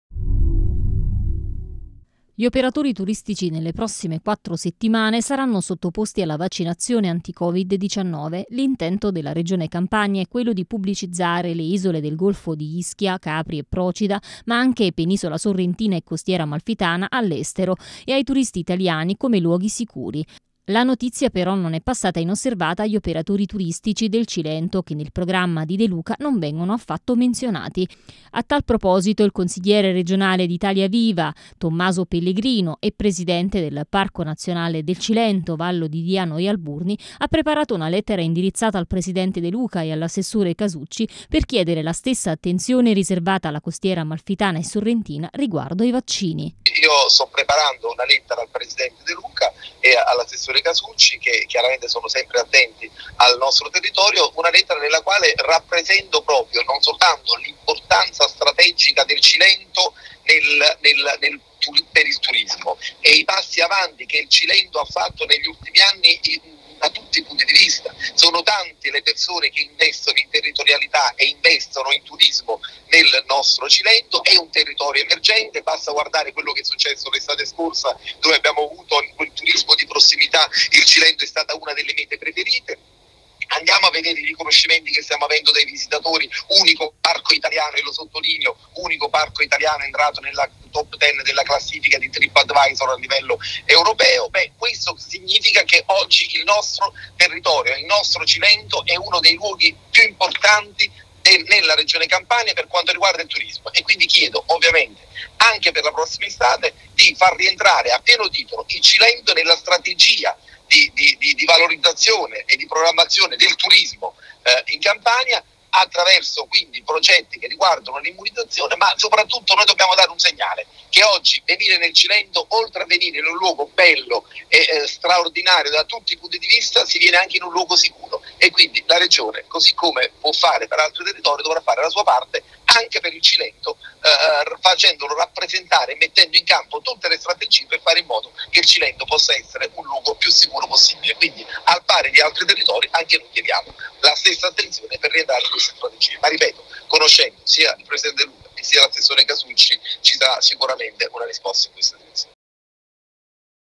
Cilento, Vaccini operatori turistici, Pellegrino:"Lettera a De Luca per lo stesso piano vaccinale"
Gli operatori turistici nelle prossime quattro settimane saranno sottoposti alla vaccinazione anti Covid-19. L’intento della  Regione Campania è quello di pubblicizzare le isole del Golfo, Ischia, Capri e Procida, ma anche Penisola Sorrentina e Costiera Amalfitana all'estero e ai turisti italiani come luoghi sicuri. La notizia però non è passata inosservata agli operatori del turistici del Cilento che nel programma di De Luca non vengono affatto menzionati. A tal proposito il consigliere regionale di Italia viva, Tommaso Pellegrino  e presidente del Parco Nazionale del Cilento Vallo di Diano e Alburni ha preparato una lettera indirizzata al presidente De Luca e all’assessore Casucci per chiedere la stessa attenzione riservata alla costiera Amalfitana e Sorrentina riguardo i vaccini.